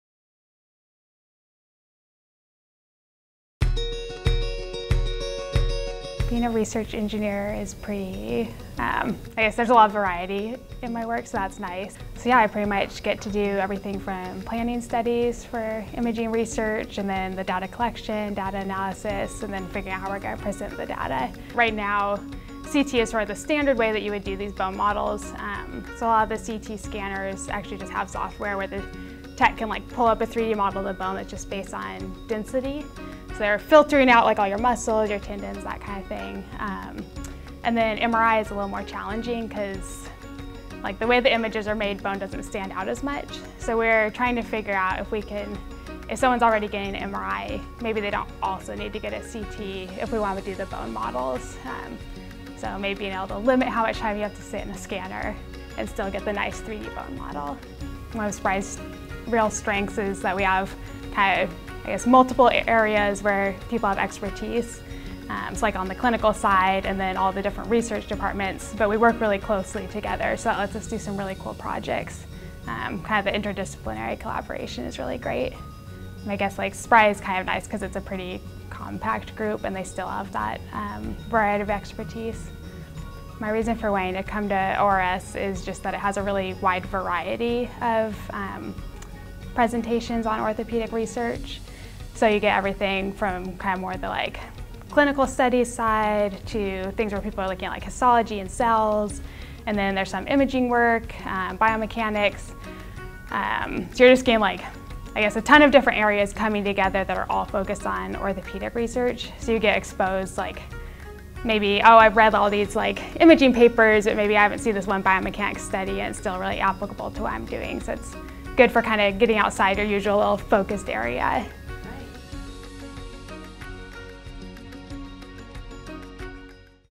ORSM 2019 Interview - Carly Lockard
We sat down with Carly Lockard, Research Engineer at Steadman Philippon Research Institute (SPRI), during the annual ORS meeting recently in Austin, Texas. "Generally, surgeons diagnose a patient looking at two dimensional images. Three dimensional images can allow additional measurements or utilization of 3-D printing. A good 3-D image of what the bone actually looks like – instead of just visual slices through the bone – can change the course of treatment." said Lockard.